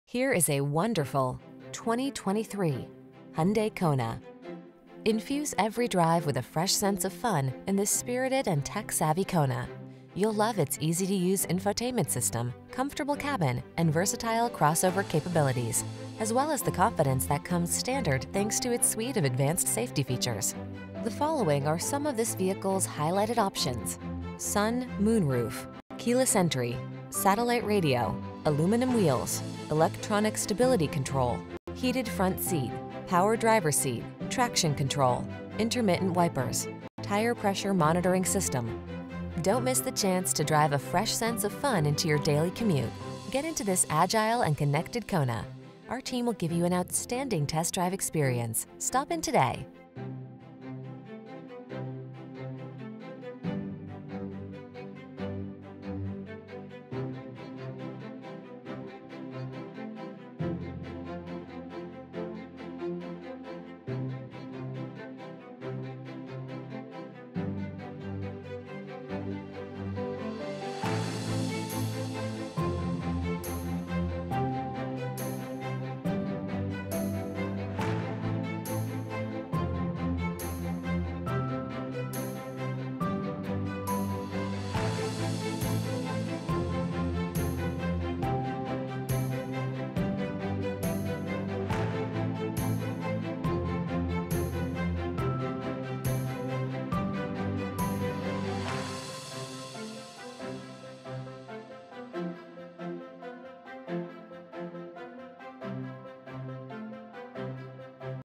2023 Hyundai Kona North Augusta Aiken Augusta Grovetown Evans U936016R
Ultra Black Pearl New 2023 Hyundai Kona SEL available in 5585 Jefferson Davis HWY, North Augusta, South Carolina at Terry Lambert Hyundai. Servicing the North Augusta, Aiken, Augusta, Grovetown, Evans SC area.

Kona SEL
2023 Hyundai Kona SEL - Stock#: U936016R

Ultra Black Pearl 2023 Hyundai Kona SEL FWD IVT 2.0L 4-CylinderbrbrRecent Arrival! 30/35 City/Highway MPG